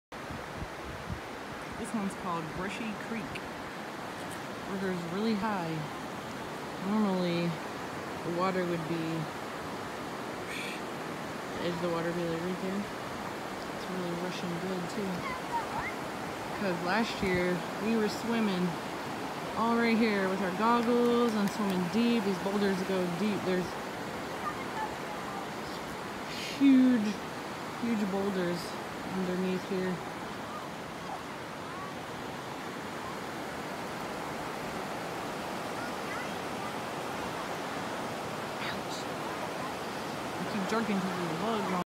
Brushy Creek, Cossatot River, AR
This swimming hole is known as Brushy Creek on the Cossatot River, AR